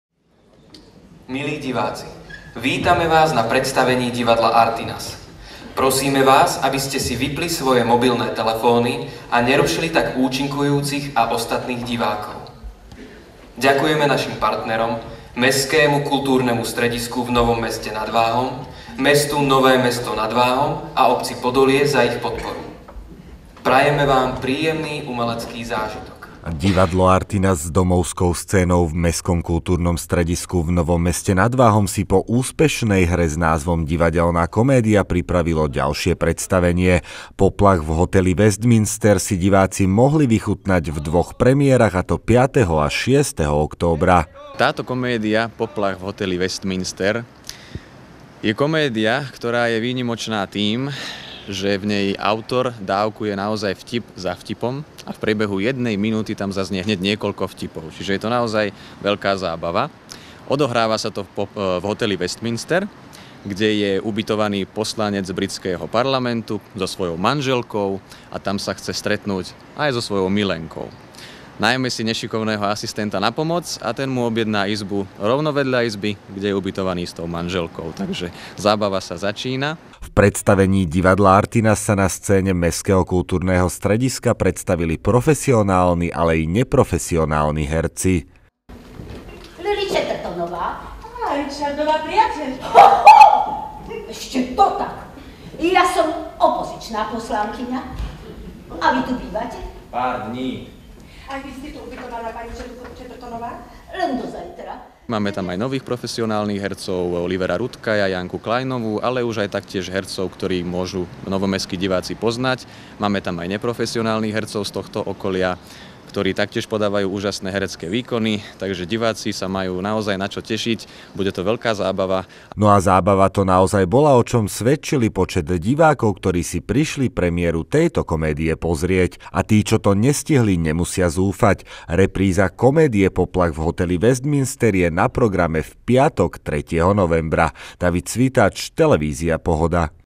PREMIÉRA NOVÉHO PREDSTAVENIA DIVADLA ARTYNAS
Divadlo Artynas s domovskou scénou v Mestskom kultúrnom stredisku v Novom Meste nad Váhom si po úspešnej hre s názvom Divadelná komédia pripravilo ďalšie predstavenie. Poplach v Hoteli Westminster si diváci mohli vychutnať v dvoch premiérach a to 5-teho a 6-teho októbra.